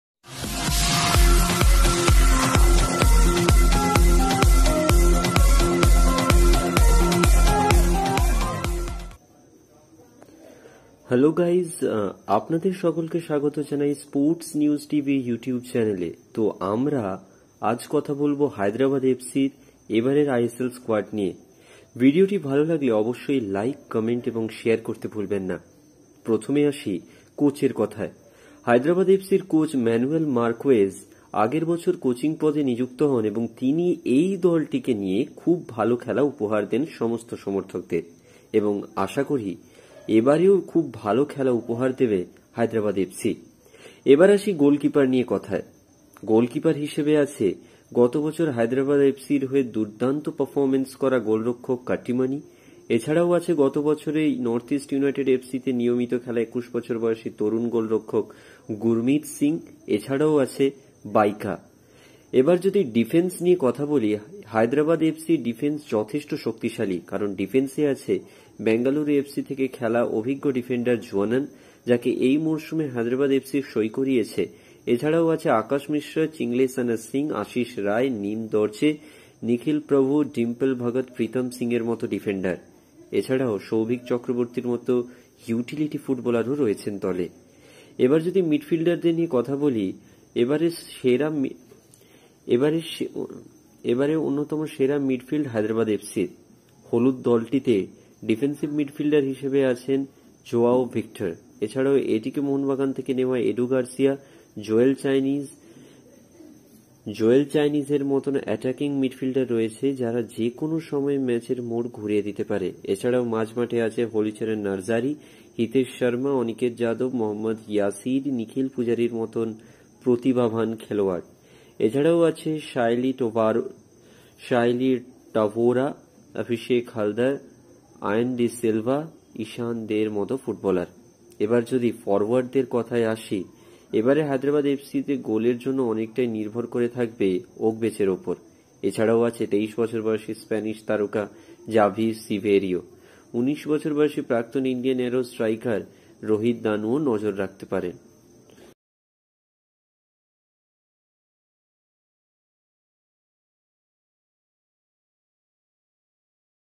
💥 কেমন হলো এবারের HYDERABAD FC দল? প্রথম একাদশ কেমন হতে পারে? রিজার্ভ বেঞ্চে কেমন? দেখুন ভিডিও...
ময়দানের খবর থেকে ভারতীয় ফুটবল সব খবর সবার আগে পেতে এখুনি  সাবস্কাইব করুন আমাদের স্পোর্টস নিউজ  চ্যানেল টিকে।।